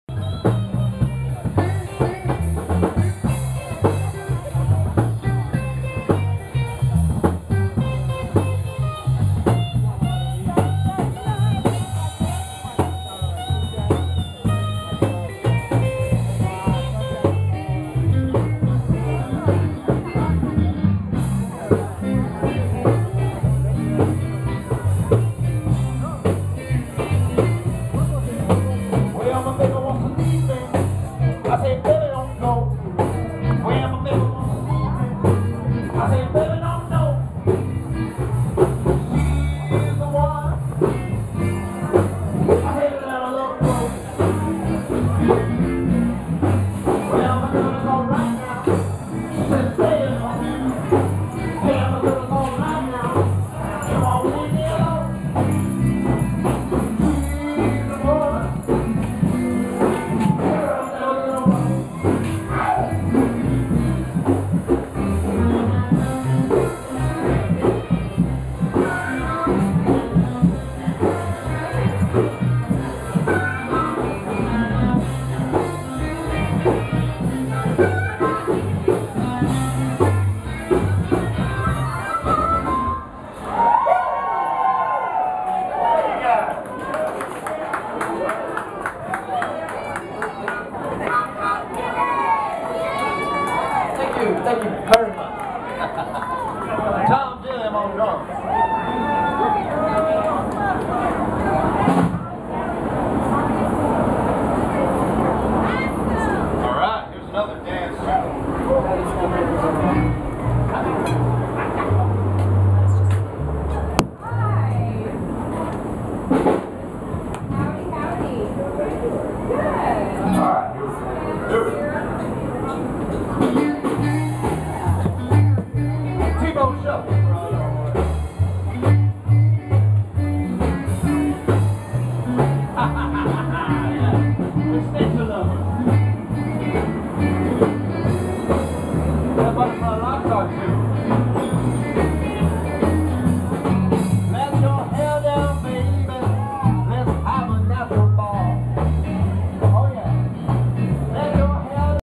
~ART SPRING 2012~ afternoon glimpse (2/2)
BA Glassworks & Tiki Lounge

Over 30 artists will set up booths offering local, accessible artwork. 5 live bands all afternoon playing music to enjoy art by.

tiki bar is BYOB for our public & guests as always.